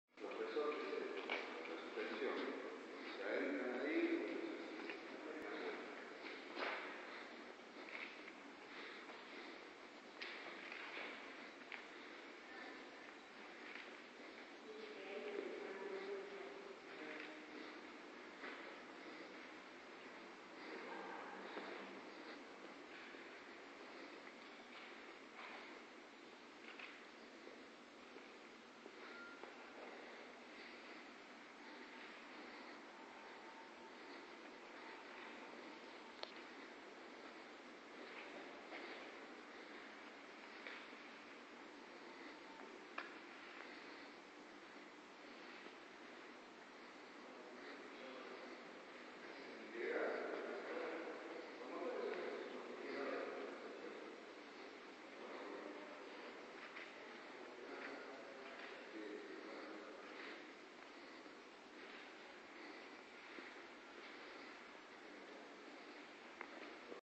Reparacion de Bogies en Emfer
En este video podran recorrer como se reparan y que se usa para reparar los bogies de unidades doble piso y simple para las diferentes lineas de Trenes dentro de EMFER ubicada en cercanias de la Estación Miguelete en la Linea Mitre